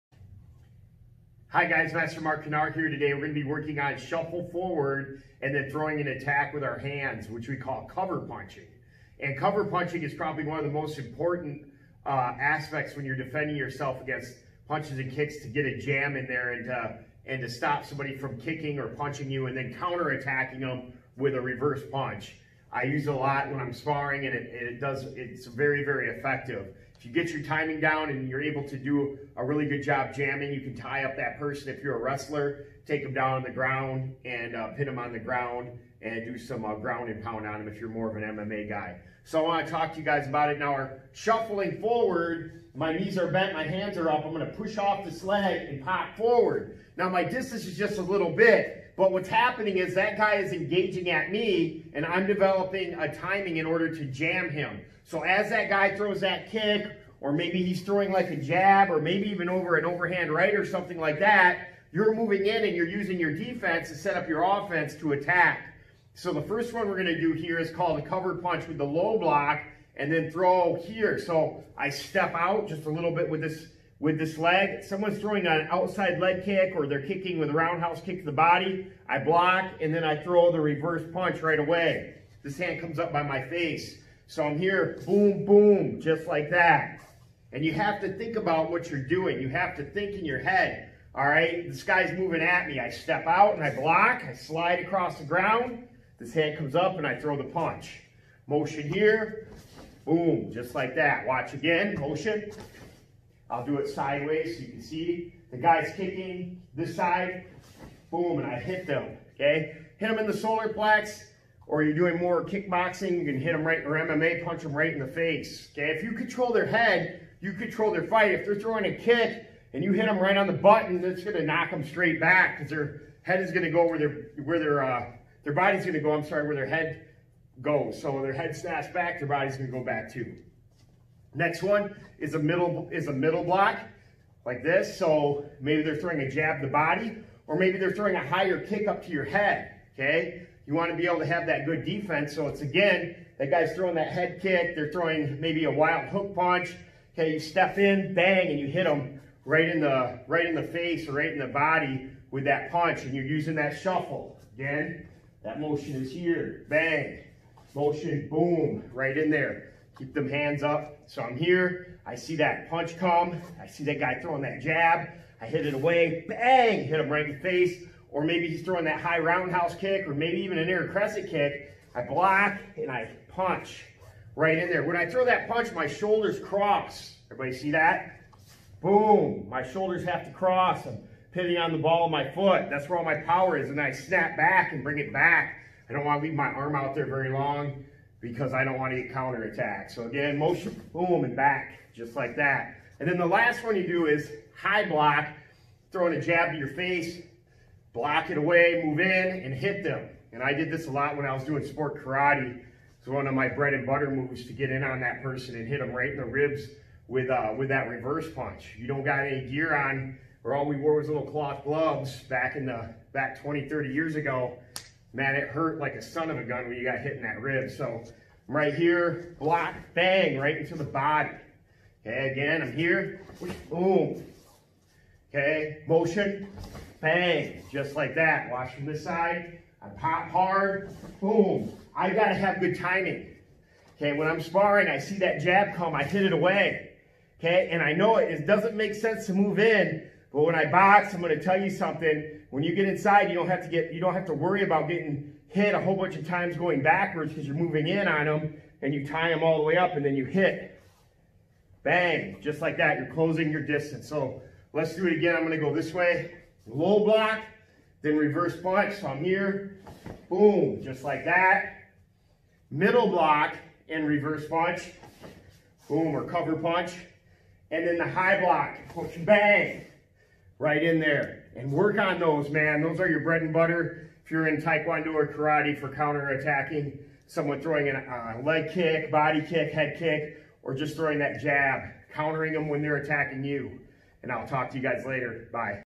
Cover Punch Set Requirement for Yellow Belt
Master Mark S. Kanaar
Che Ko Taekwondo Academy
Grey Wolf Martial Arts
Yellow Belt Requirement
Cover Punch Set
Shuffle Forward Low Block then Reverse Punch
Shuffle Forward Middle Block then Reverse Punch
Shuffle Forward High Block then Reverse Punch
Train Hard and Enjoy!!!